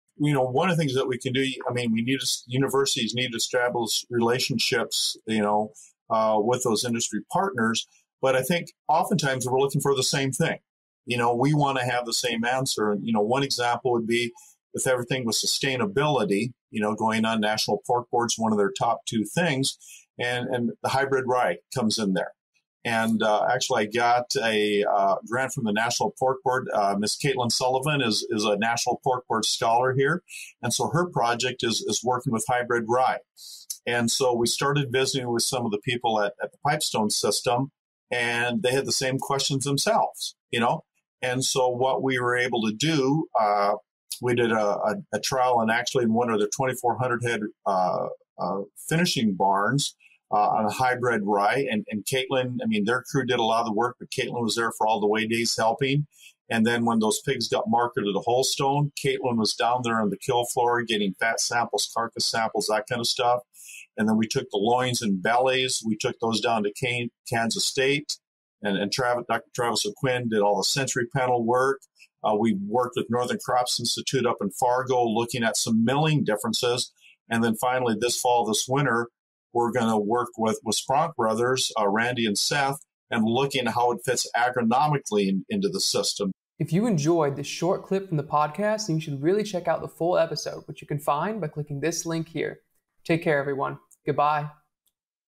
Universities and industry collaborating toward the same goals in research
After graduating from South Dakota State University with a bachelor's and master's degree, Dr. Thaler earned his PhD from K-State. After that, he returned to SDSU to work as a swine extension specialist, teacher, and administrator. In this episode, he talks about collaboration and networking - how to improve the industry together. Interesting, right?

The Swine it Podcast Show is trusted and supported by innovative companies like: